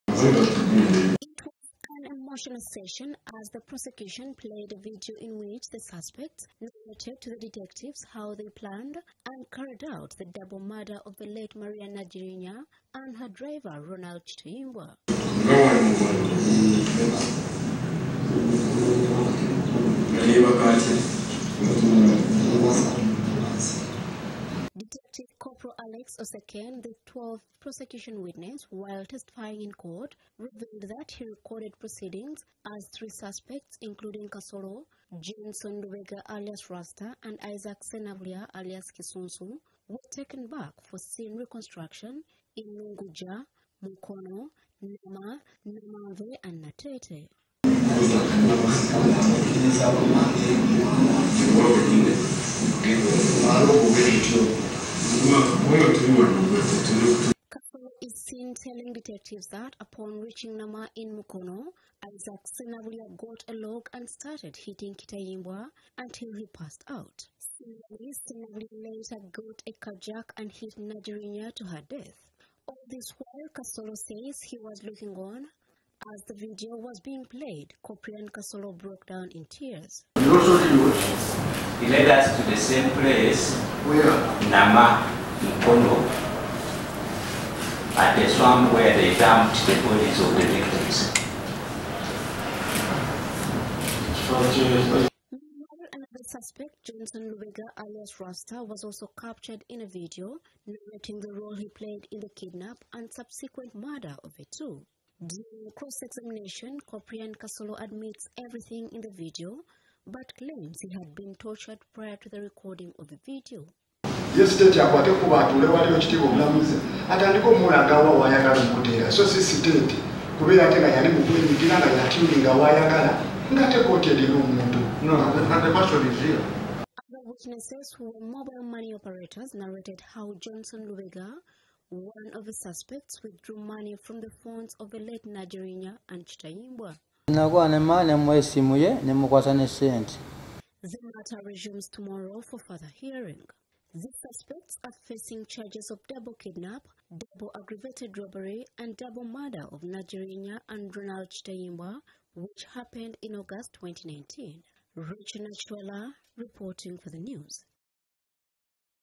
Maria Nagirinya murder case
Relatives and friends of the late Maria Nagirinya and Ronald Kitayimbwa were left in tears as they watched a video in which suspect Coprian Kasolo narrated how he and others murdered them, before dumping their bodies in a trench.

The emotional video left those present in shock; even the suspect was seen weeping.

In 2019, Maria Nagirinya was kidnapped just outside the gate of her home in Busega Lungujja, together with her driver. Their bodies were discovered in Mukono.